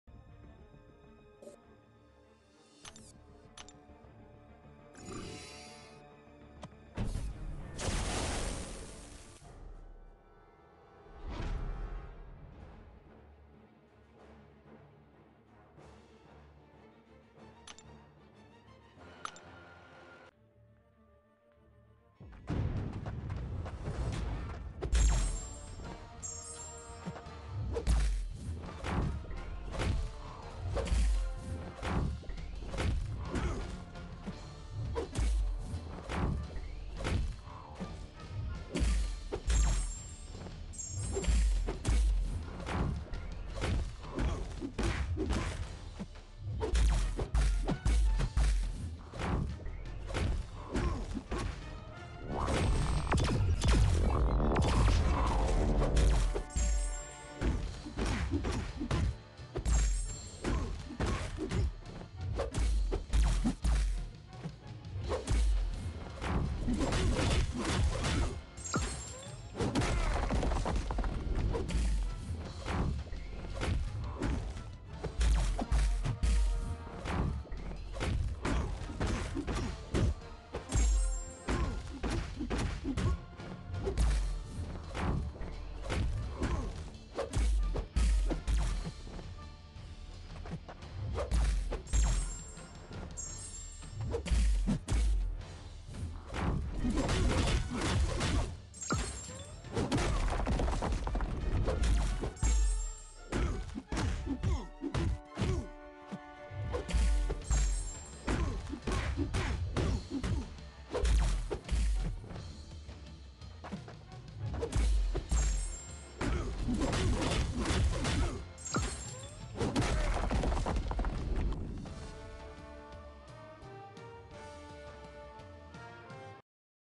AW Tier 2 - Havok on Node 24 (EMP Modification)
Warlock vs Havok on Node 24 - EMP Modification in Alliance War.